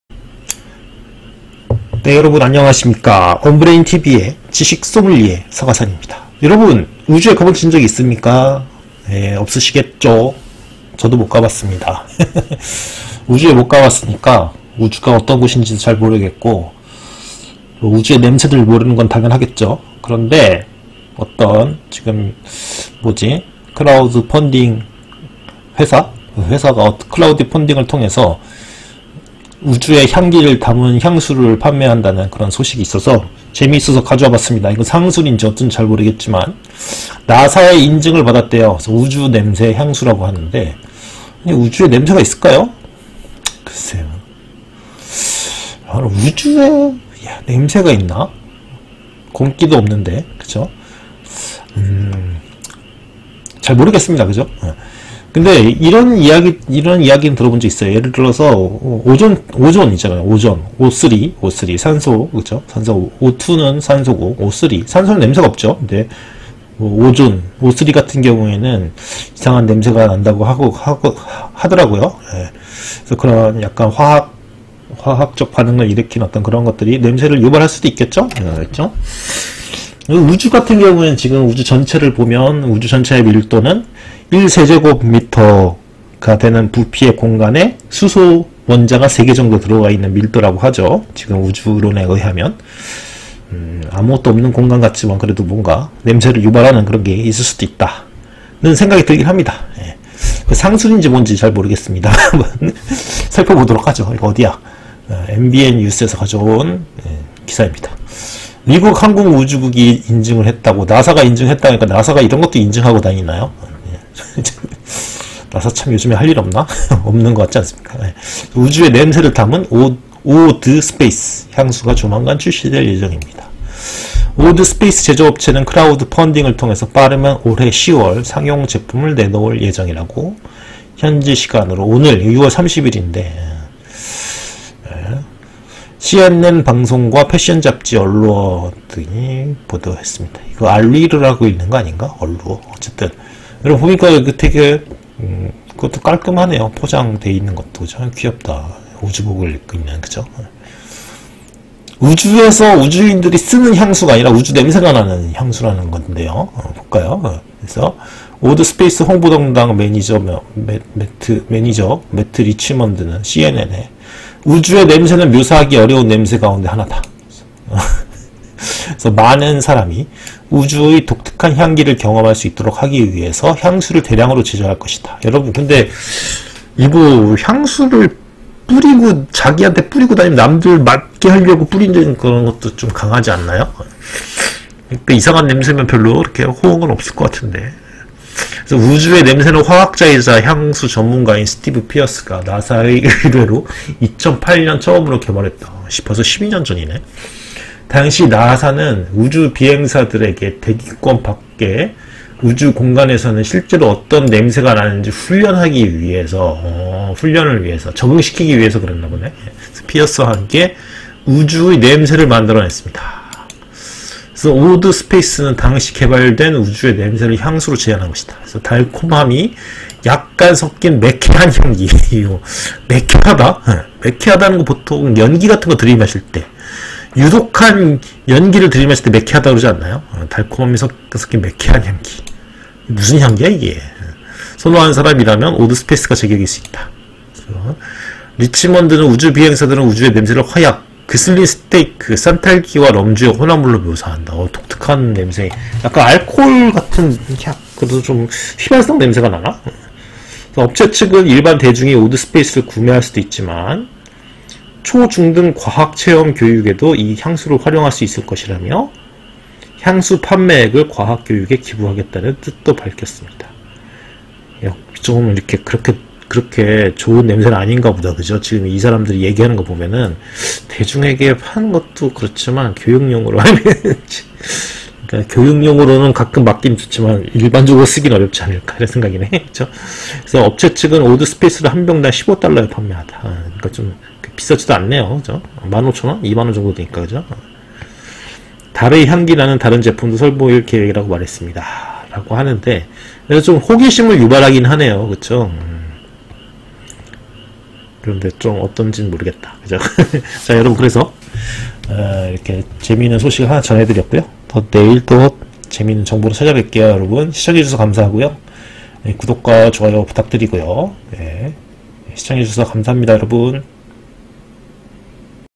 우주는 어떤 냄새가 날까?
그걸 구현한 우주 향수가 있다고 합니다! 짱신기한데요~ 한번 알아보시죠^^